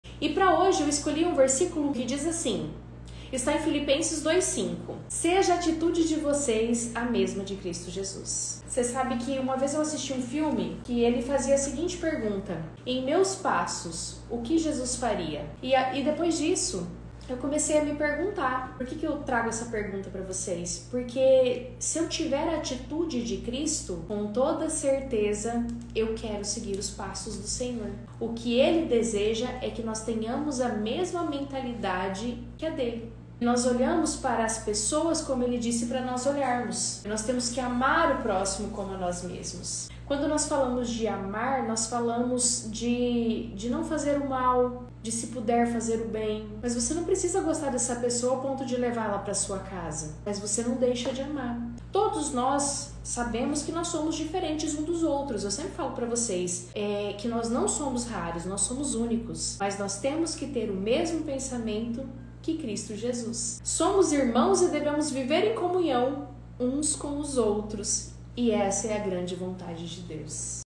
Lapidando em Fé - Filipenses 2:5 | Marina Feld
Para que este domingo seja muito abençoado com a palavra do Senhor, eu escolhi o versículo que está em Filipenses 2:5 que diz o seguinte: “Seja a atitude de vocês, a mesma de Cristo Jesus”.

Na Bíblia está escrito que nós devemos fazer o bem e amar o próximo, assim como Jesus Cristo amou e perdoou à quem o feriu. E quando nós trazemos essa palavra para a nossa realidade, como Profissionais de Estética, devemos entender que nosso objetivo de vida é ajudar as nossas pacientes, ao invés de prejudicá-las.

Sabe quando você atende uma paciente que tem melasma e olha apenas a mancha no rosto dela? Isolada e como se não tivesse relação com mais nada do corpo desta paciente? Nesse momento você não está seguindo os passos de Cristo, porque não vai cuidar do individuo como um ser triuno, que tem alma, corpo e espírito.

Nós, Profissionais de Estética integrativas, trabalhamos olhando o corpo dos pacientes de dentro para fora, na raiz do problema. Então se hoje você, que está aí do outro lado, não tem resultados satisfatórios em seus tratamentos de melasma, que tem uma fila de pacientes insatisfeitas, entenda que a partir do momento que você seguir os passos de Cristo, todas as coisas boas acontecerão na sua vida.

O que eu falei no vídeo e escrevi na legenda, complementando a ideia central, faz sentido para você? Deixa aqui nos comentários a sua opinião!